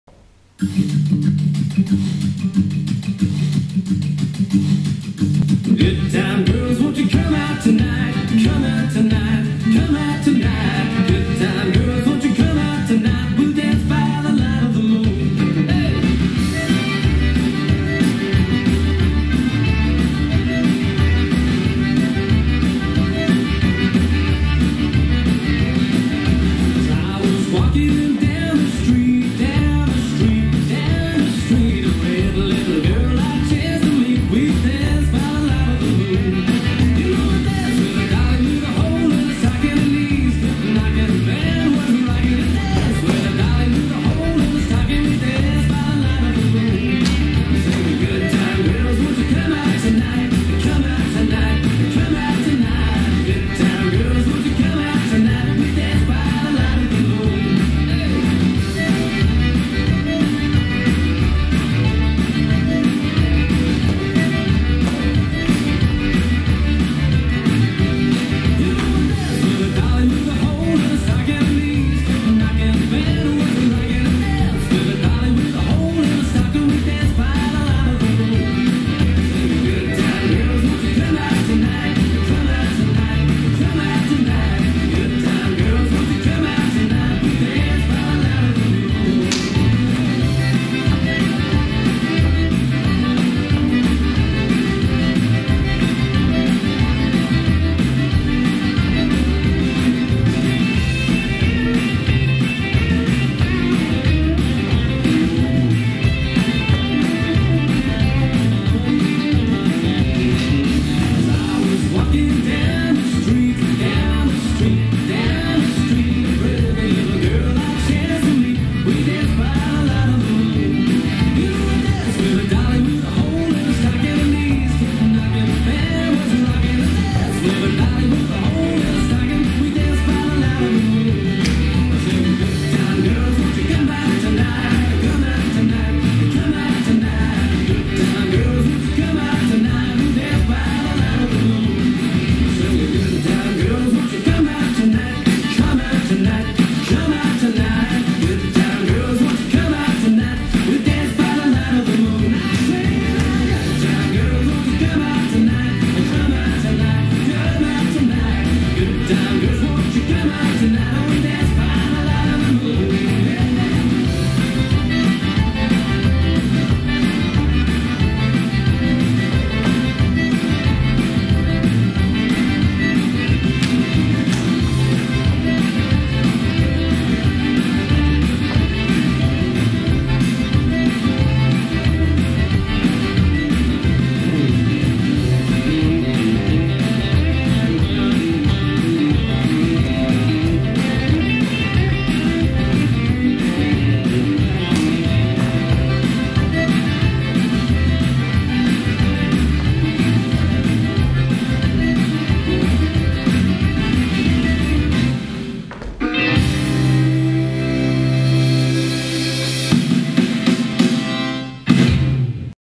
Good Time Girls - Karen Kennedy
32 count, 4 wall line dance
no tags/restarts
Danced by Karen Hannaford